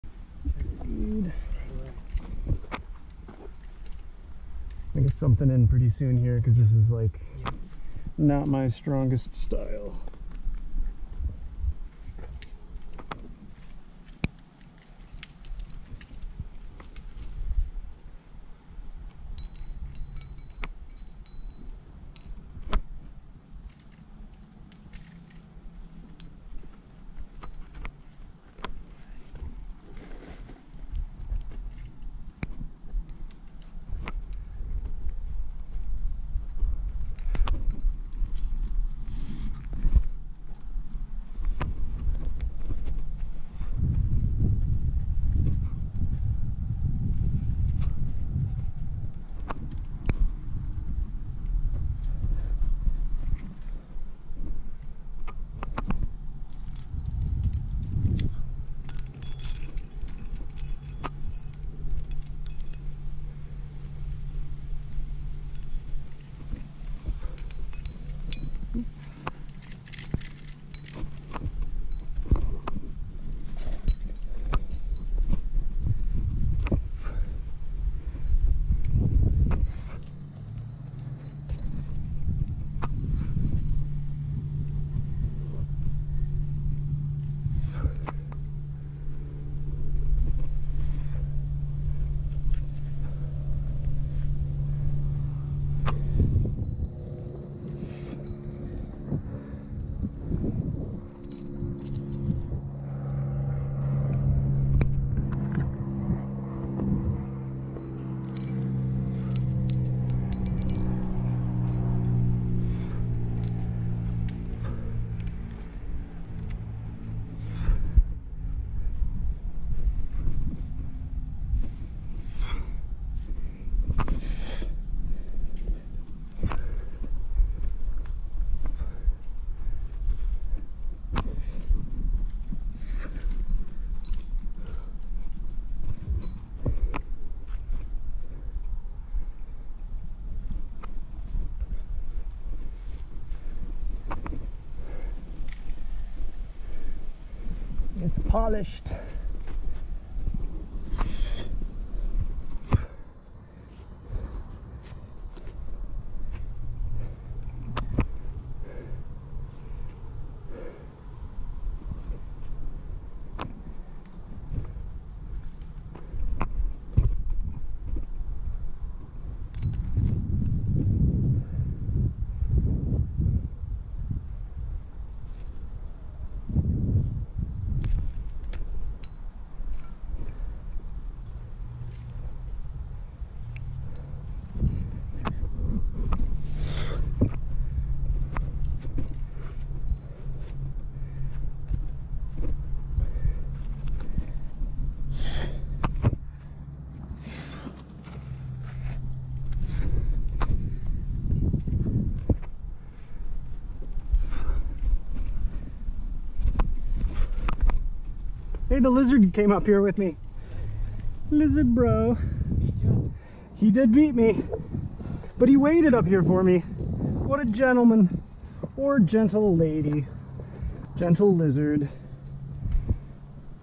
Clean Starts - 5.8 Neat and Cool, Squamish British Columbia Canada
Some finger crack practice for me. Granite finger crack is sooo good!